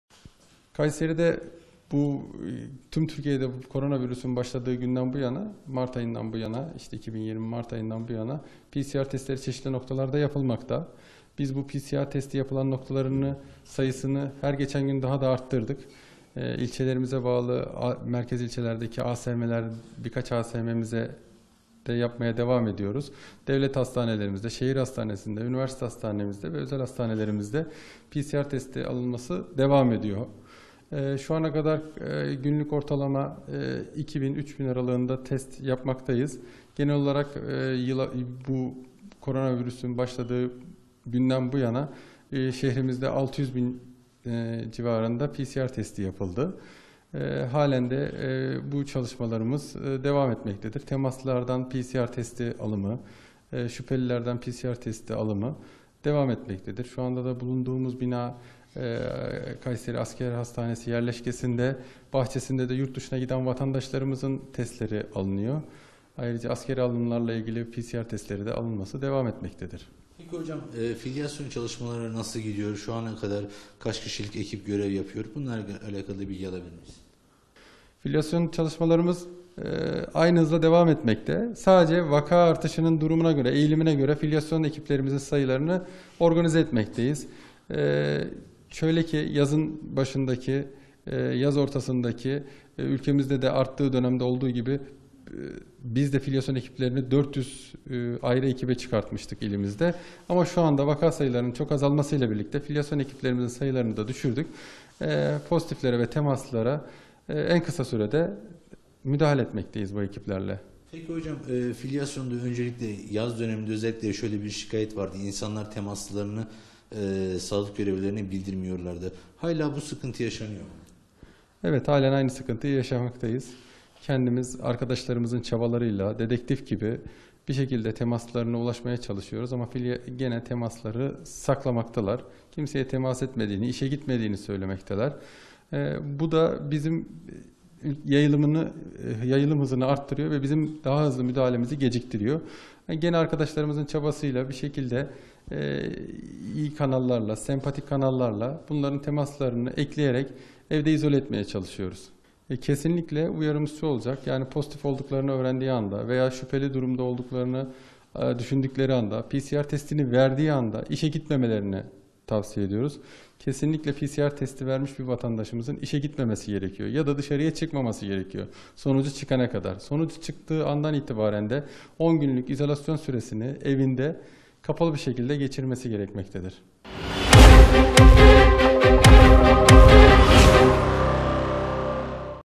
'Arkadaşlarımız dedektif gibi çalışarak temaslıları bulmaya çalışıyor'
KAYSERİ İl Sağlık Müdürlüğü Halk Sağlığı Hizmetleri Başkan Yardımcısı Burhan Akın, filyasyon çalışmalarında yaşadıkları zorluğa değinerek, "Arkadaşlarımız kendi çabaları ile dedektif gibi çalışarak temaslıları bulmaya çalışıyor. Ancak, bazı insanlar temaslılarını saklamaya devam ediyor" dedi.

Kayseri İl Sağlık Müdürlüğü Halk Sağlığı Hizmetleri Başkan Yardımcısı Burhan Akın, koronavirüs salgınına karşı yapılan çalışmalarla ilgili açıklamalarda bulundu. Salgının başladığı günden bu yana PCR testlerinin çeşitli noktalarda yapıldığını belirten Akın, "PCR test merkezi sayılarını her geçen gün daha da çok artırdık. Aile Sağlığı Merkezi, Devlet Hastanesi gibi birçok hastanede PCR testi yapılıyor. Günlük ortalama 2 ile 3 bin kişi aralığında PCR testi yapıyoruz. Koronavirüsün başladığı günden bu yana da 600 bin civarında PCR testi yapıldı. Halen bu test çalışmaları devam ediyor. Temaslı, şüpheli kişilerden testleri alıyoruz. Yurt dışına gidecek, askere gidecek vatandaşlarımız yerleşkemizde sürüntü ile PCR testi oluyor" dedi.'BAZI İNSANLAR TEMASLILARINI SAKLAMAYA DEVAM EDİYOR'

Akın, kentte filyasyon çalışmalarının aralıksız devam ettiğini, vaka durumunun eğilimine göre ekip sayısının değiştiğini söyledi. Yaz ortasında filyasyon ekip sayısını artırdıklarını belirten Akın, şöyle konuştu:

"Pozitif ve temaslı olan kişilere en kısa zamanda ulaşmaktayız. Filyasyon ekiplerimiz, pozitif vaka olan insanlarımızın temaslı kişileri söylemesinde halen sıkıntı yaşamaktadır. Koronavirüse yakalanan insanlar isim vermekten kaçınıyor. Arkadaşlarımız kendi çabaları ile dedektif gibi çalışarak temaslıları bulmaya çalışıyor. Ancak, bazı insanlar temaslılarını saklamaya devam ediyor. İnsanlar kimseye temas etmediğini, işe gitmediğini söylüyorlar. Bu durum yayılım hızını artırıyor. Bizim müdahale hızımızı geciktiriyor. Vatandaşlarımız pozitif olduklarını öğrendiklerinde ya da şüpheli duydukları zamanda PCR testi versinler ve işe gitmesinler. Sonuç açıklanana kadar dışarı çıkmasınlar. Sonuç çıktığı andan itibaren 10 gün evde beklemeliler."